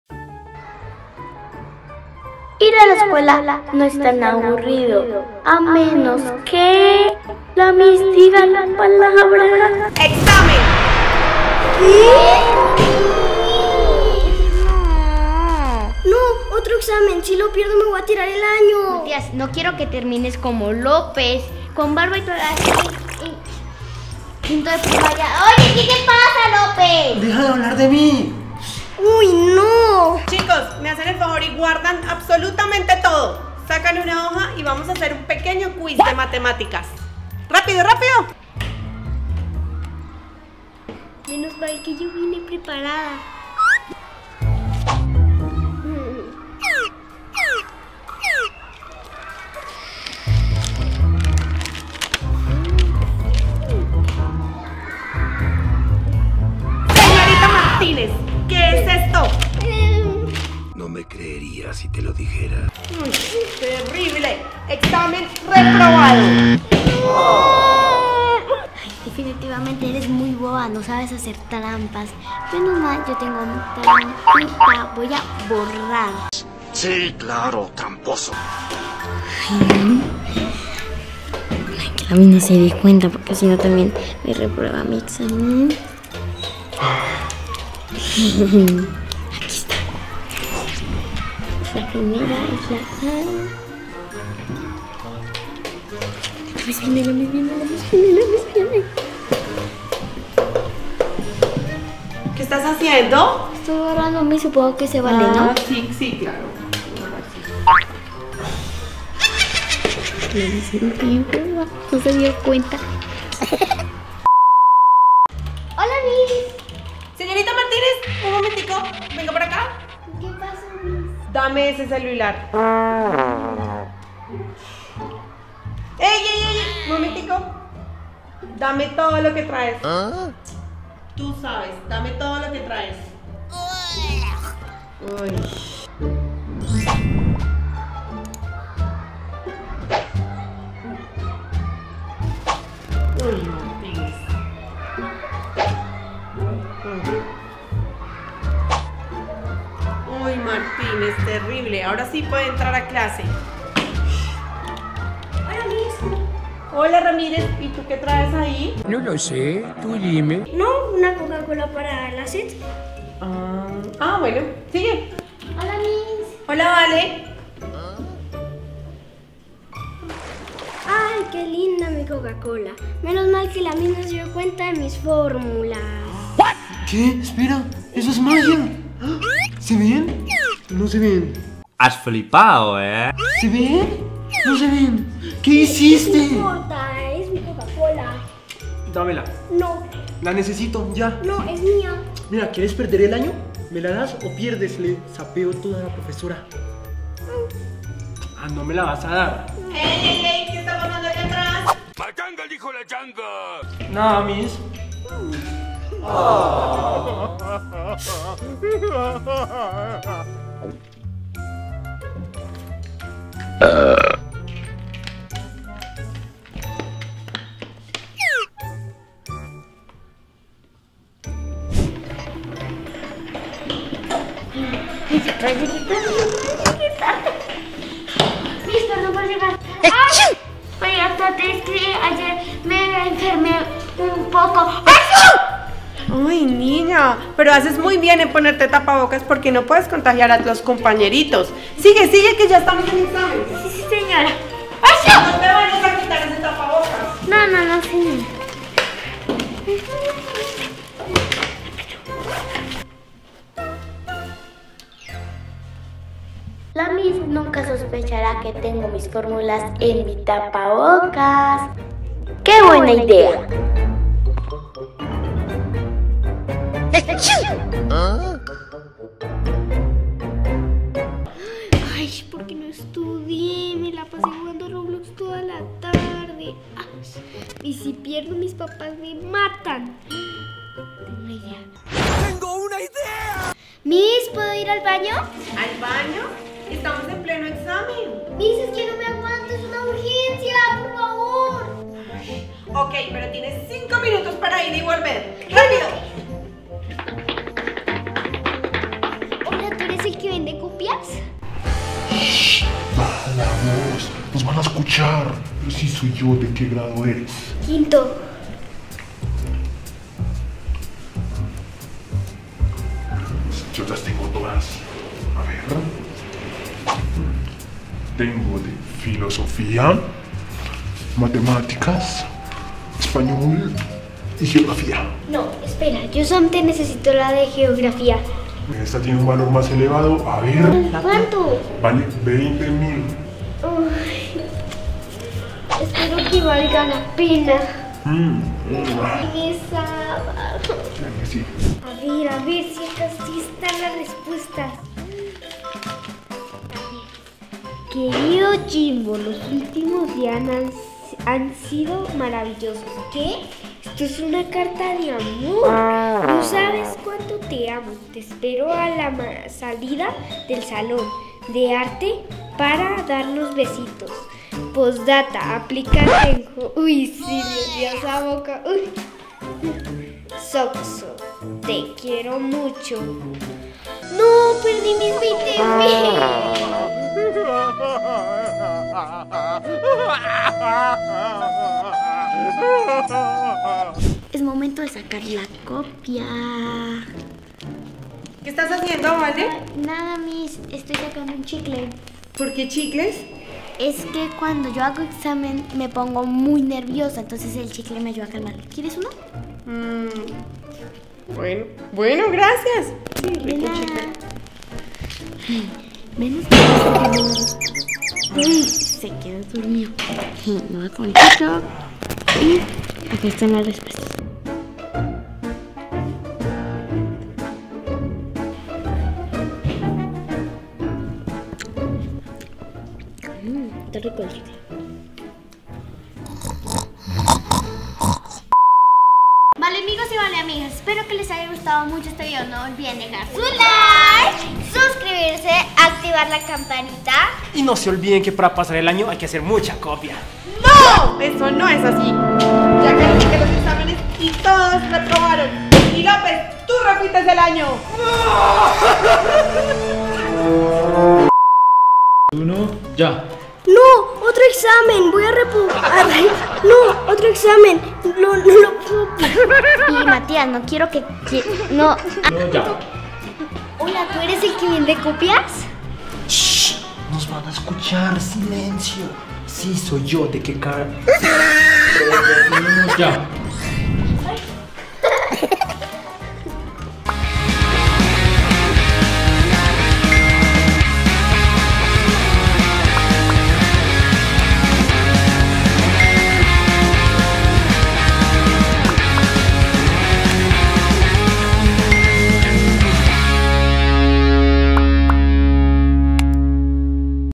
TRUCOS ESCOLARES SECRETOS 🤫 || Cómo hacer TRAMPA en La ESCUELA
🦄Hola Valeamig@s La palabra EXAMEN es lo peor que podemos escuchar en La Escuela, pero con algunos divertidos Trucos podemos Sobrevivir a ellos y pasar la materia... Reprobar no estará en tus planes A+.

Comenta que TRUCOS sabes y compártelos con los Valeamigos y Valeamigas 😉

Recuerden que LOS AMOO DEMASIADO!! 💕

**LEO TODOS LOS COMENTARIOS ASI QUE DEJAME EL TUYO... BESOS!!!**